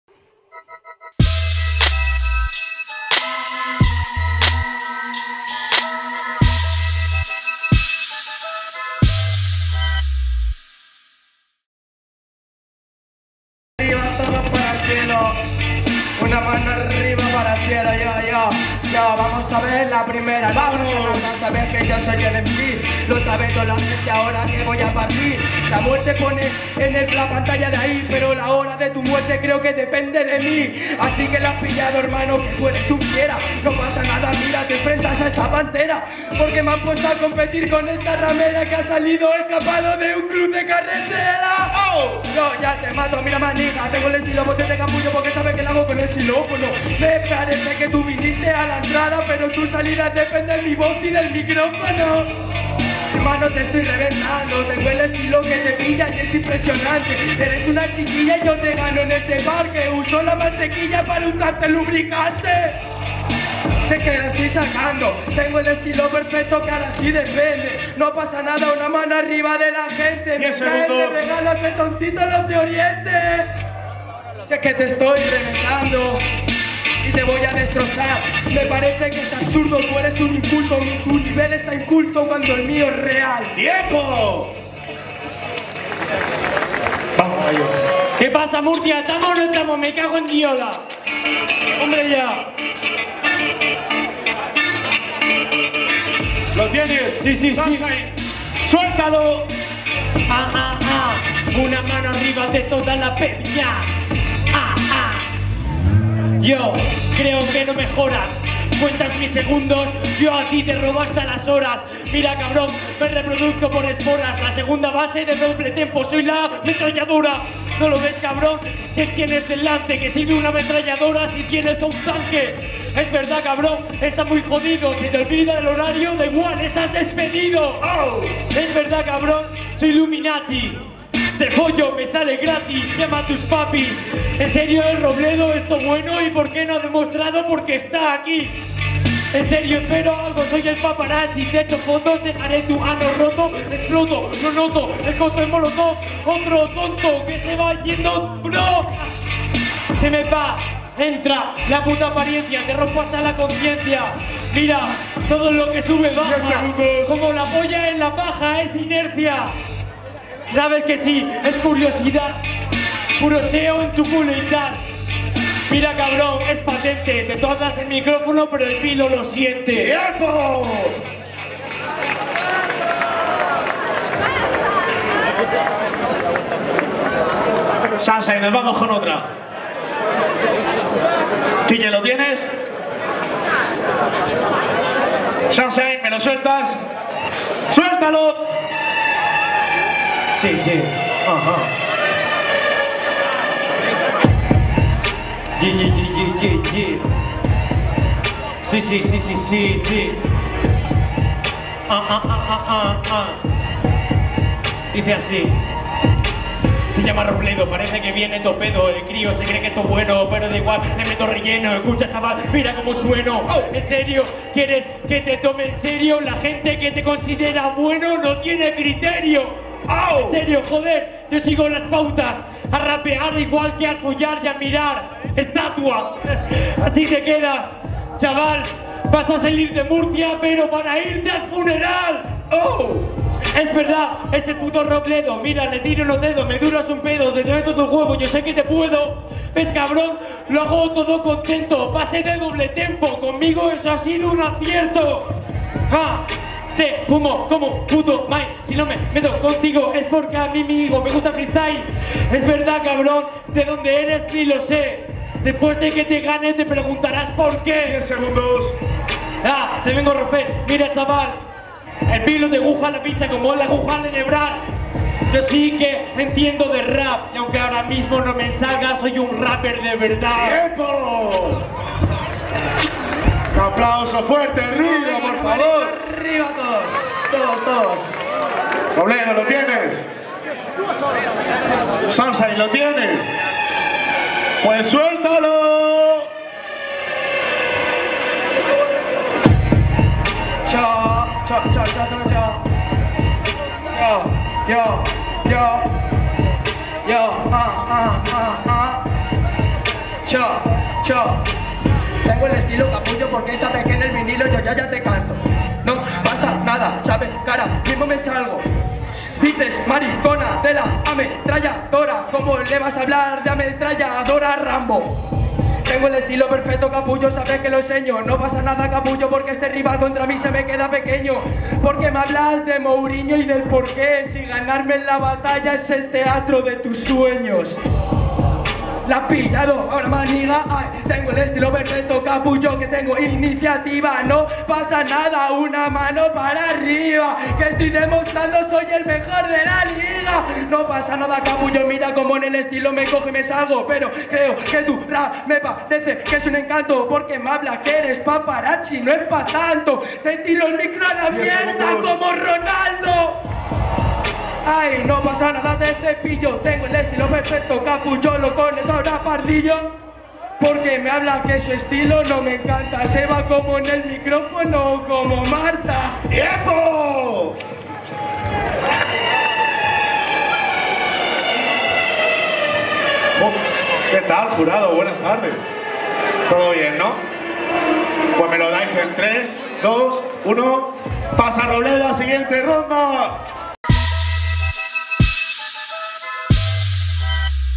ROBLEDO vs VILO / OCTAVOS / BDM MURCIA 2017
OCTAVOS BDM REGIONAL MURCIA 2017.
robledo vs vilo. gana robledo
Si quieres ser tú el que mañana este en ese escenario, empieza ya.